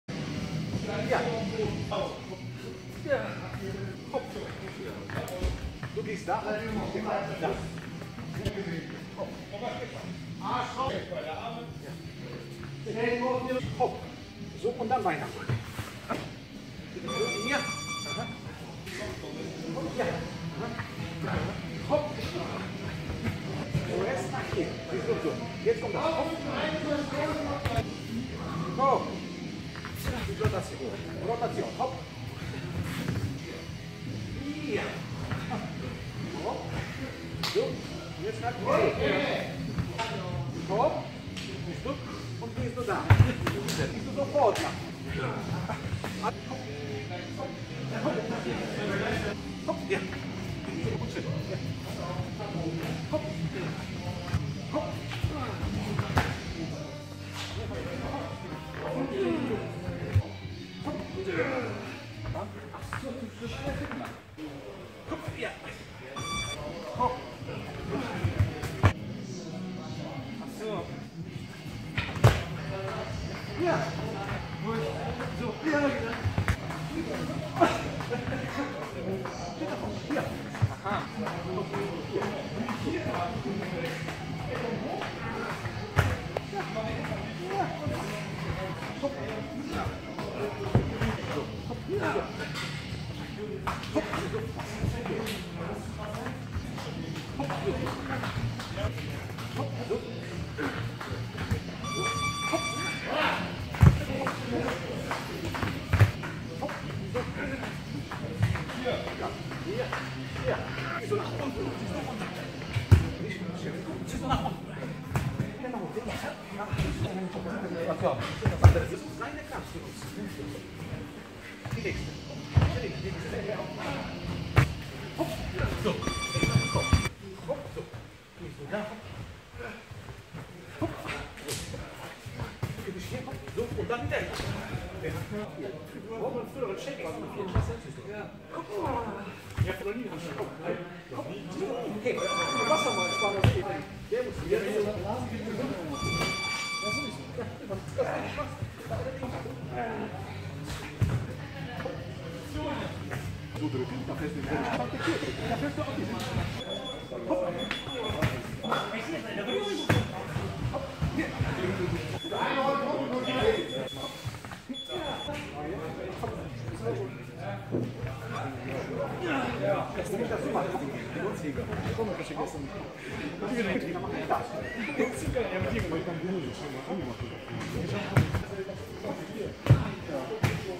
Self Defense Trailer 1.2
Qin Na - Nahkampf & Intuitive Selbstverteidigung
Trainingsausschnitte

Hallo ich bin „Karol“

Ich begrüße Sie und freue mich sehr, dass Sie sich mein YouTube Video ansehen.

Das was man hier sehen kann, sind nicht nachgestellte Trainingsausschnitte, die zeigen sollen, was ich anzubieten habe.
Was ich mache ist eine Nahkampf & Selbstverteidigung Interpretation auf Basis meiner praktischen Erfahrung und der Kenntnisse über die menschliche Anatomie, Vitalpunkte, die biomechanischen Prinzipien, Psychologie und menschliche Automatismen.

MfG Karol